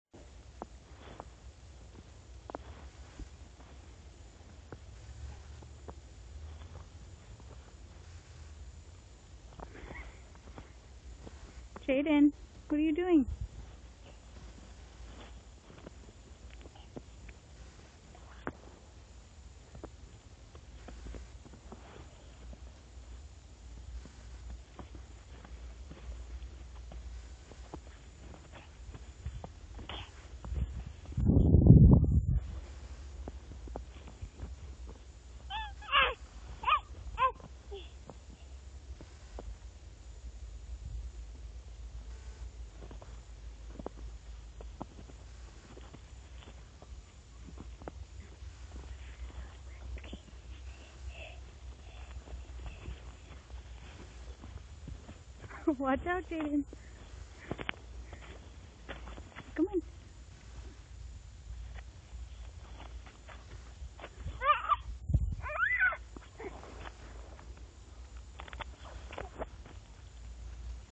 Snow baby
Jaden playing in the snow